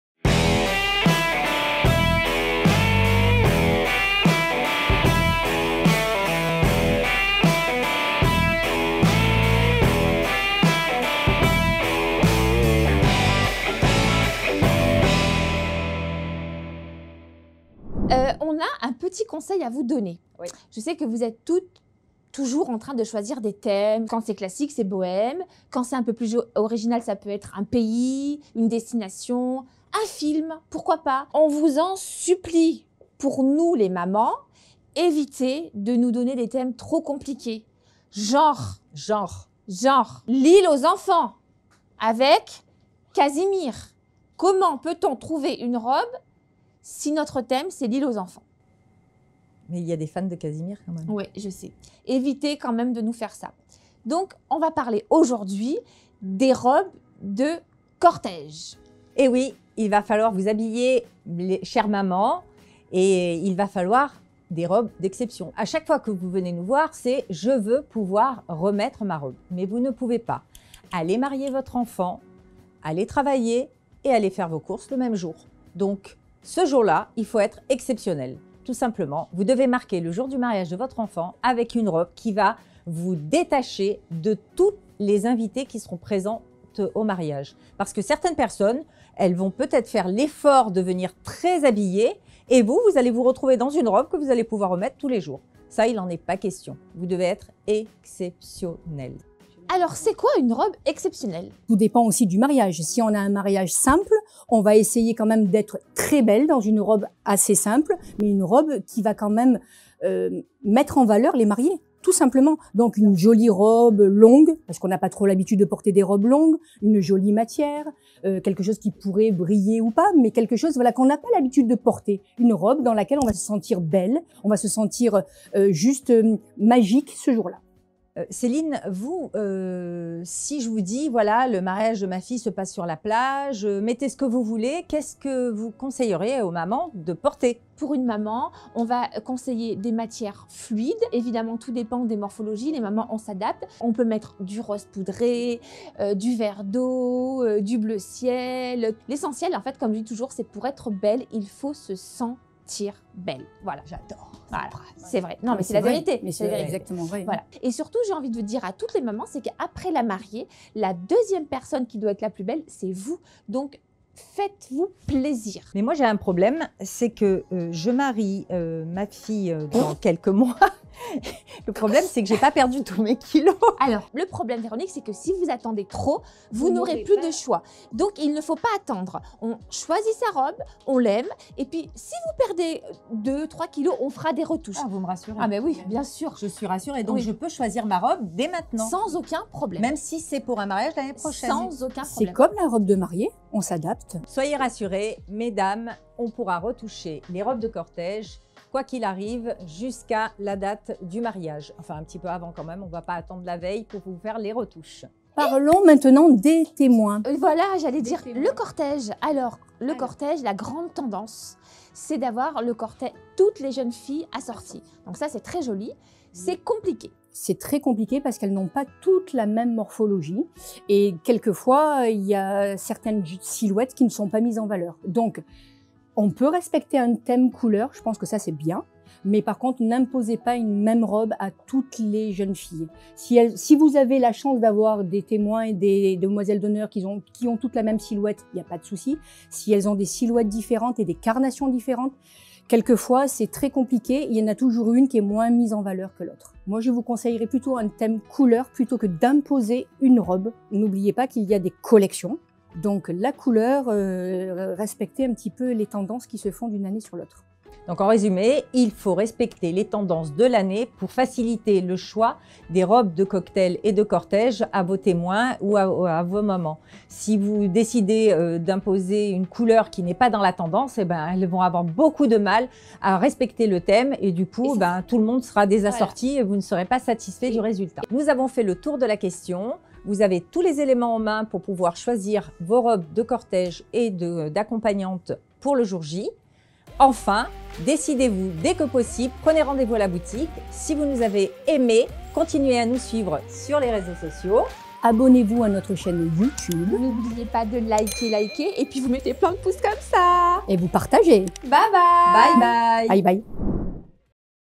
ÊTRE EN ACCORD AVEC LE THÈME DE SON MARIAGE ?!
Choisir un thème pour son mariage n'est pas chose aisé, faire correspondre tout le monde avec encore moins ! Voici nos conseils pour être en parfait acord avec le thème de votre mariage.

À Dimanche prochain !